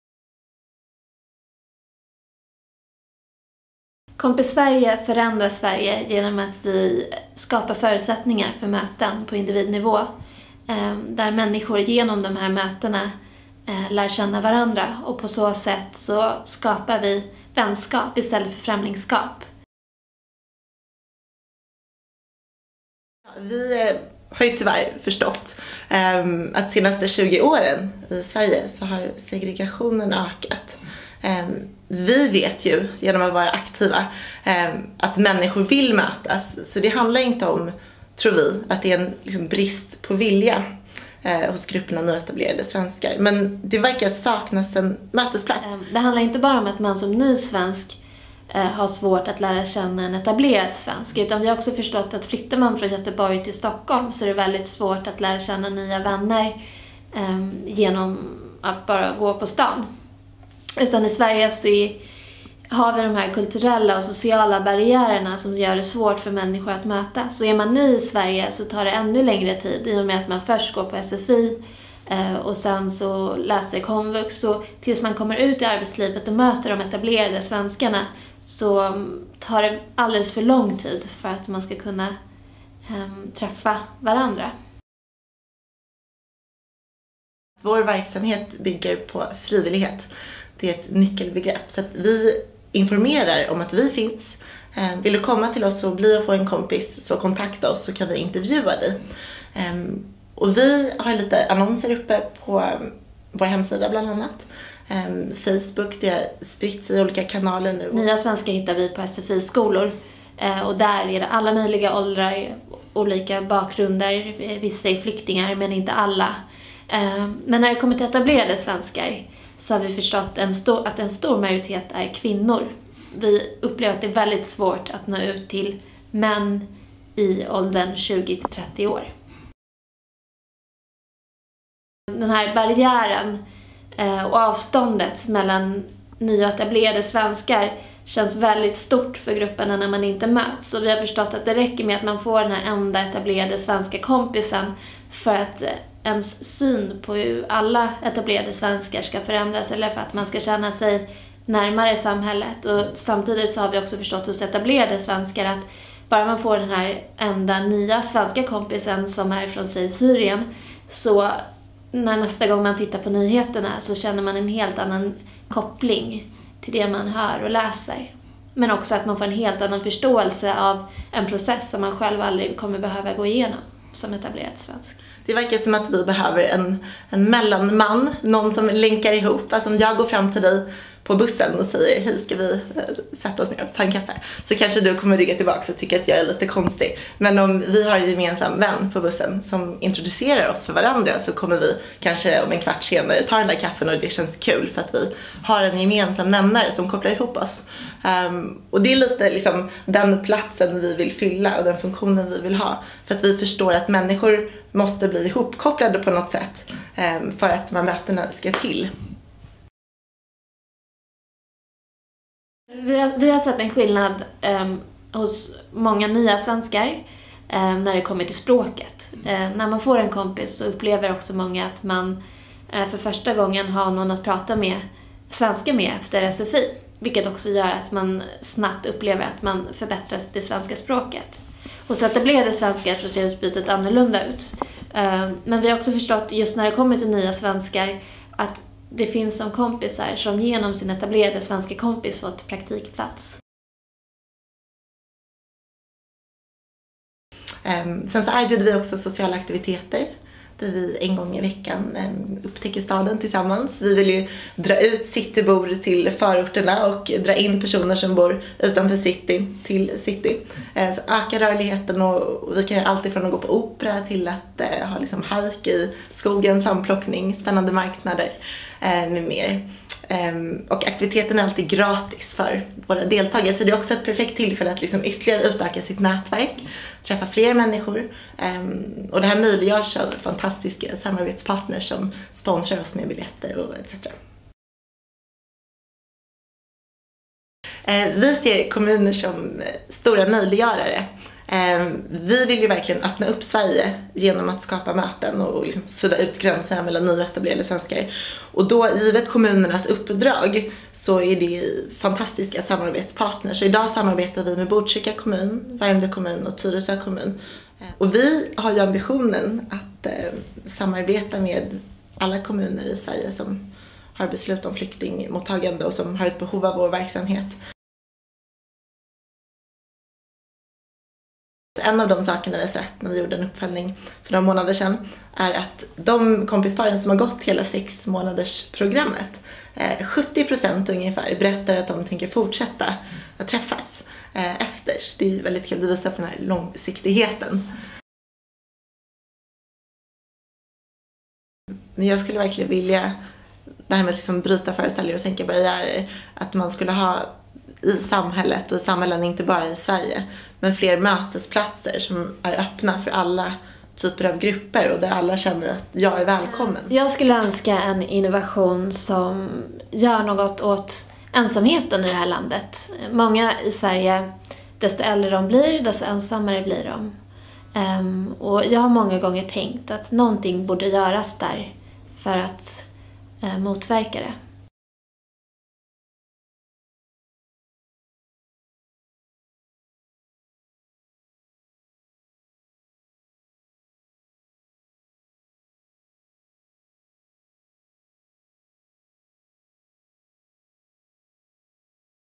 Kompis Sverige – integration på individnivå
Kompis Sverige hjälper människor från olika bakgrunder att mötas. Genom att matcha ihop människor som nyligen kommit till Sverige med redan etablerade svenskar initieras vänskapsrelationer, med målet att skapa ett Sverige utan vi och dom. Kompis Sverige arbetar med integration på individnivå och har startats av Pegah Afsharian och Natassia Fry.

Kompis Sverige är en del av Axfoundations projekt ÖppnaDörren som syftar till att bredda nätverk mellan etablerade och nya svenskar.